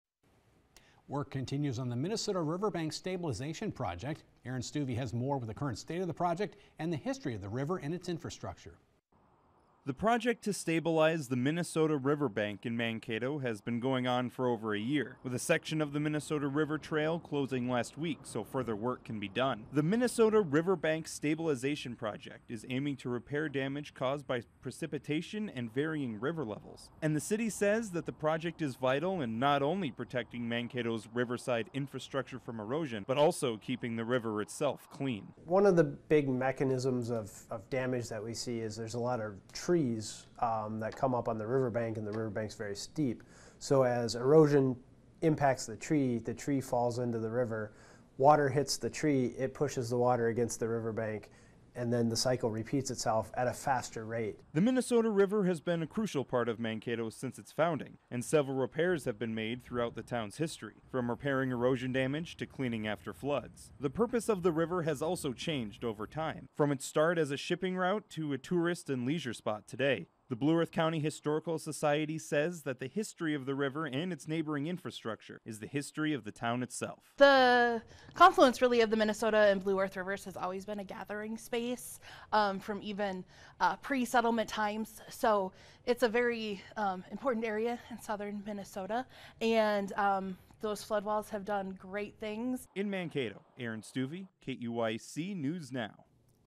Riverbank Stabilization project continues
The project to stabilize the Minnesota River bank in Mankato has been going on for over a year , with a section of the Minnesota River Trail closing last week s